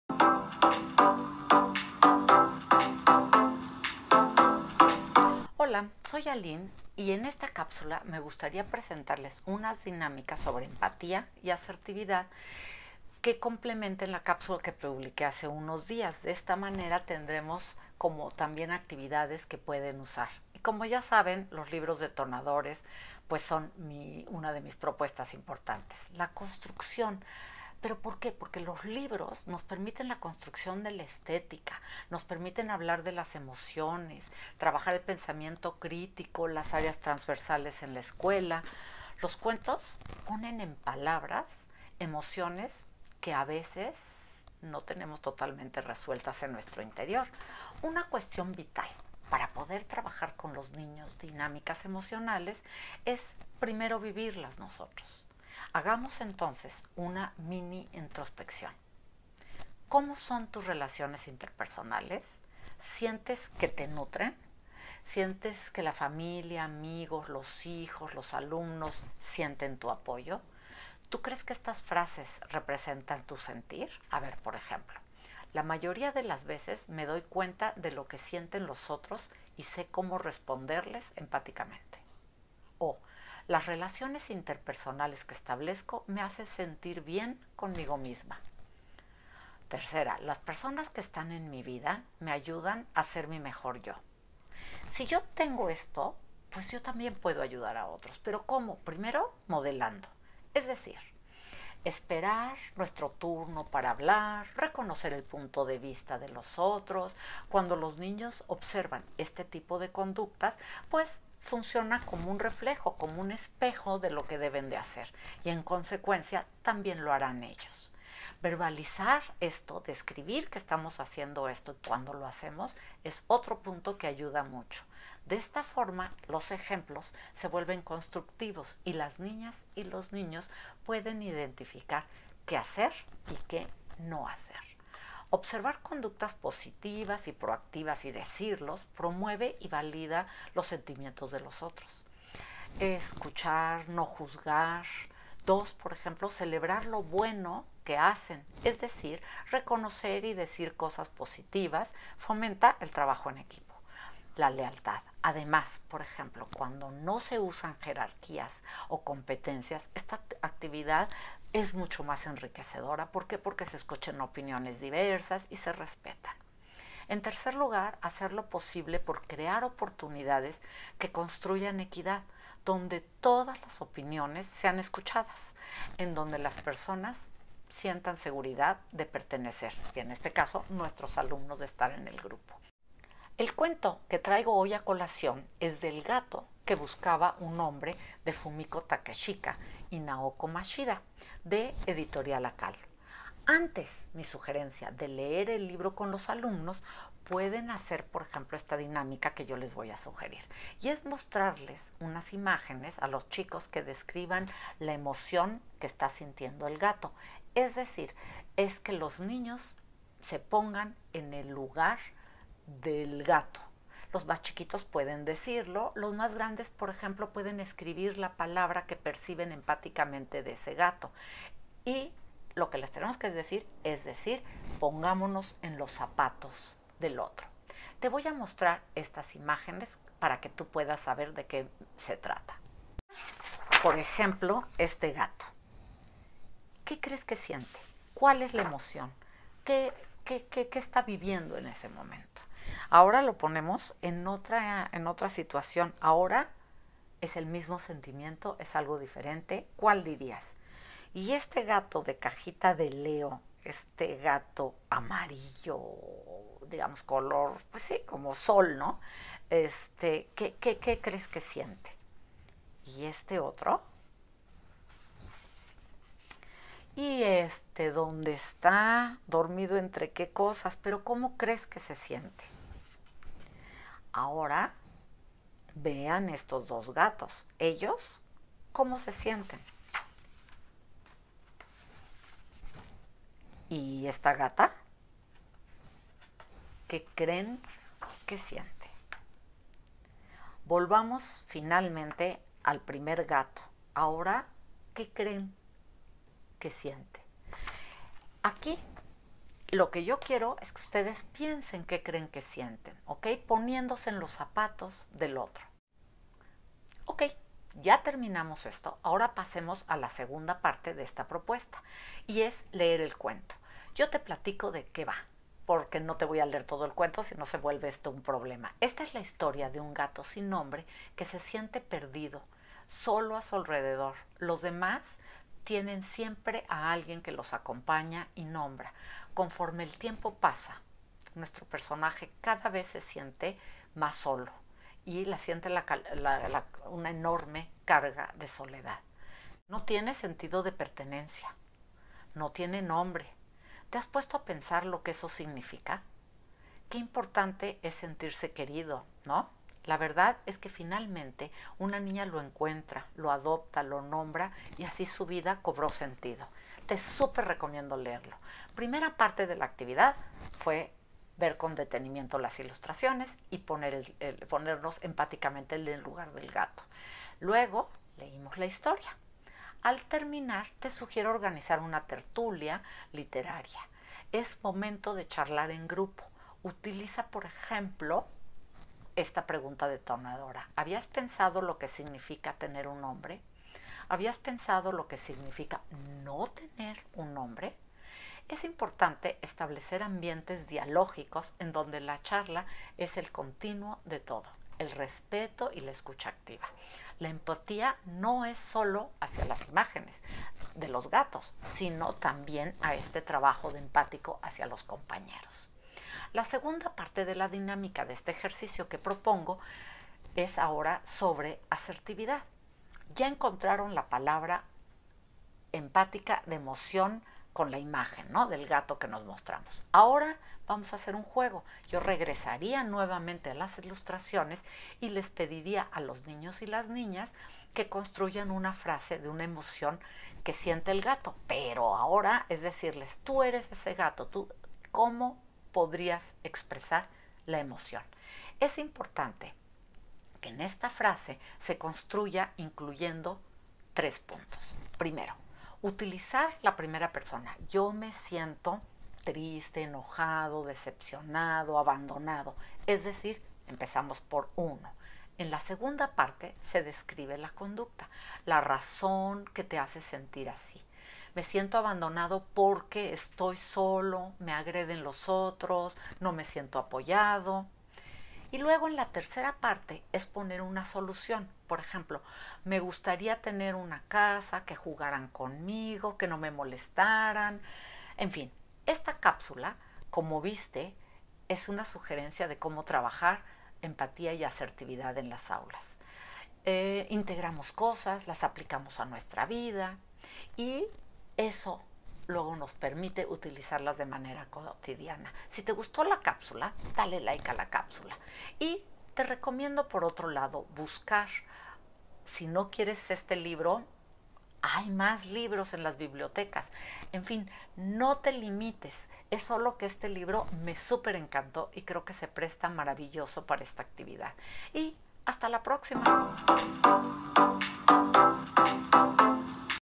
Dinámica empatía y asertividad
En esta cápsula se ofrece una dinámica que abarca empatía y asertividad para trabajar en las aulas.  A través de un libro se sugiere hacer estas dinámicas.